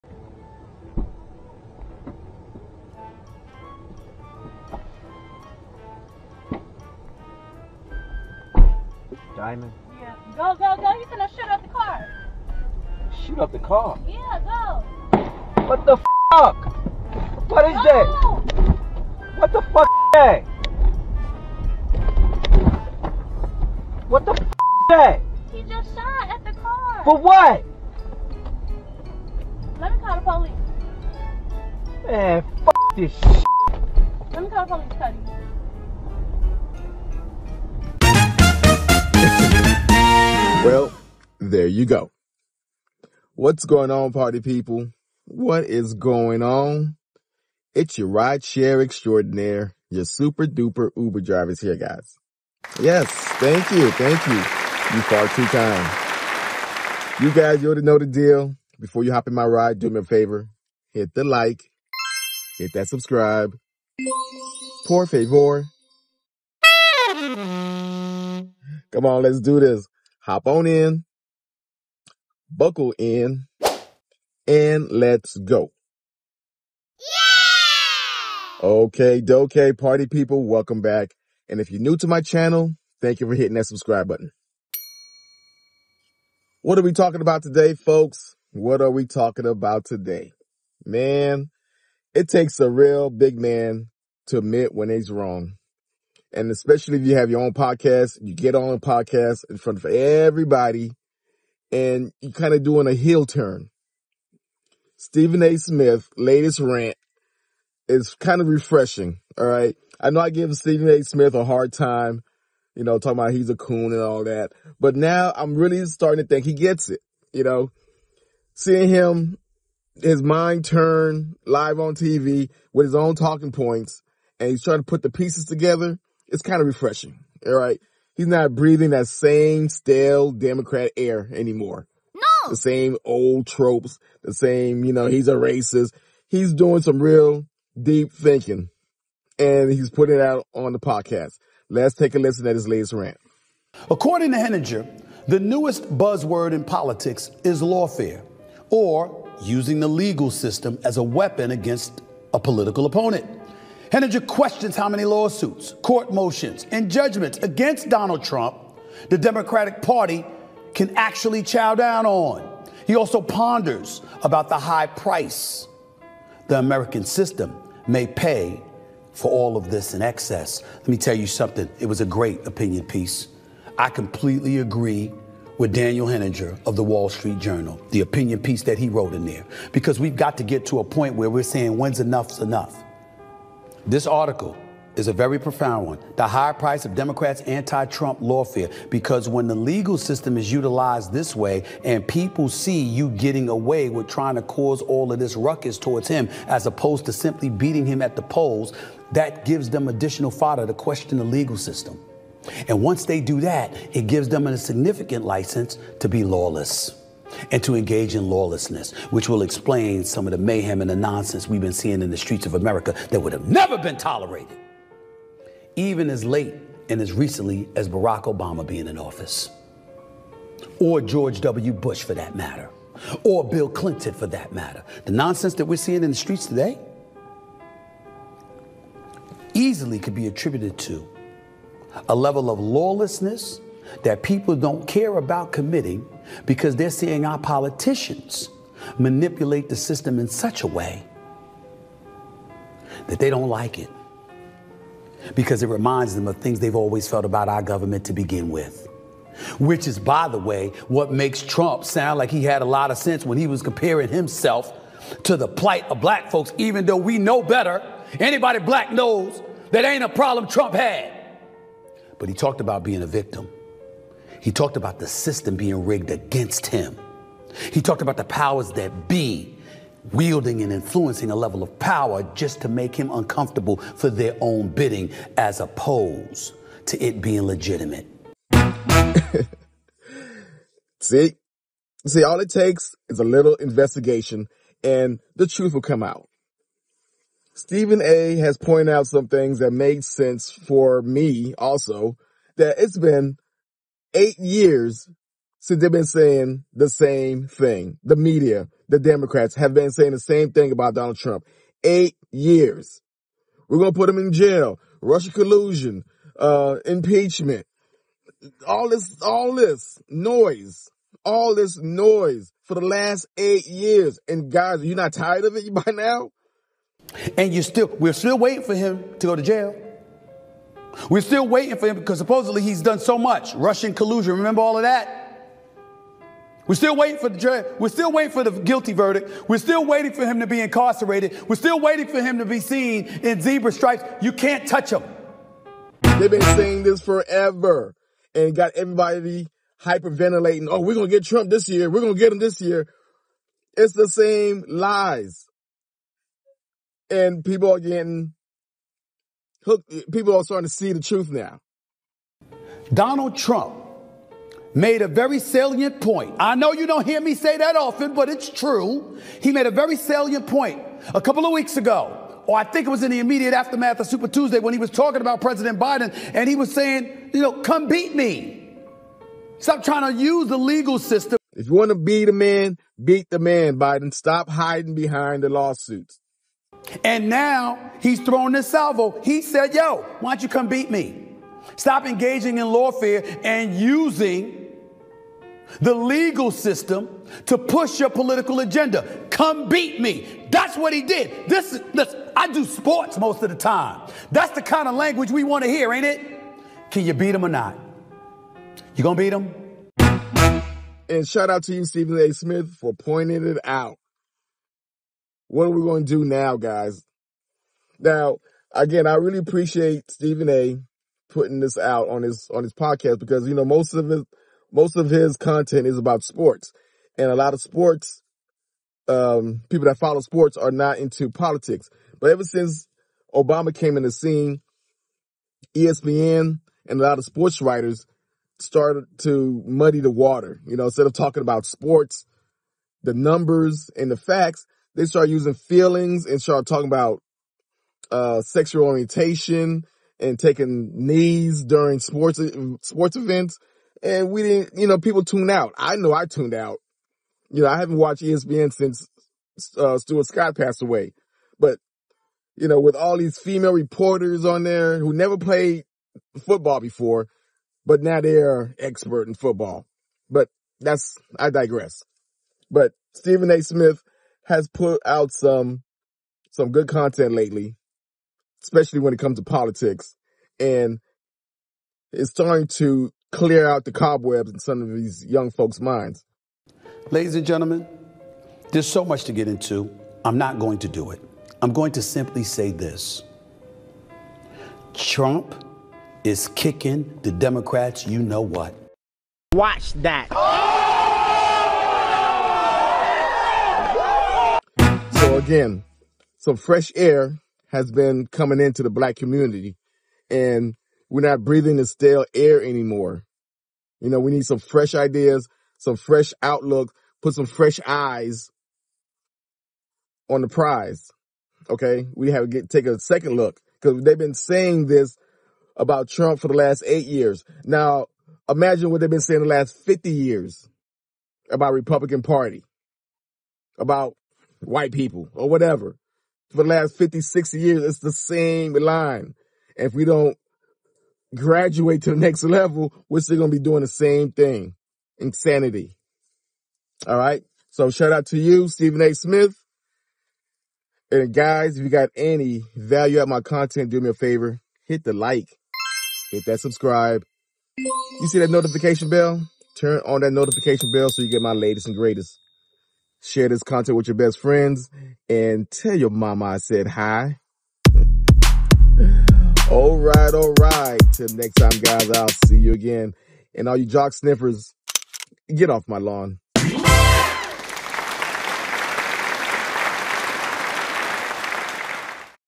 Shocking Truth Behind Stephen A. Smith's Lawfare Critique by Democrats
Shocking Truth Behind Stephen A. Smith's Lawfare Critique by Democrats. Stephen A. Smith's recent challenge to Democrats highlights concerns over perceived bias in legal actions against Trump. Smith advocates for fairness in what he terms "lawfare," calling for equitable treatment in legal proceedings. His stance fuels ongoing debates about the intersection of politics and justice in high-profile cases.
00:00 Uber in Chiraq
02:22 Epiphany
05:49 Come beat me
07:58 Kicking butts